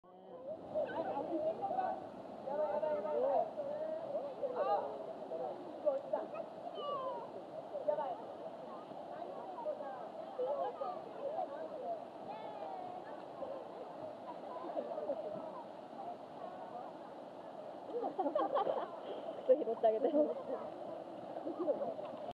鮎川 川遊び(11)
2016夏？秋？まだまだ夏は終わりませんよ！！
友達にナイススポットを教えて貰った！
今月中には飛び込めるようになります！
それにしても、橋からバク中決めたお兄さんかっこよすぎです(  ；∀；)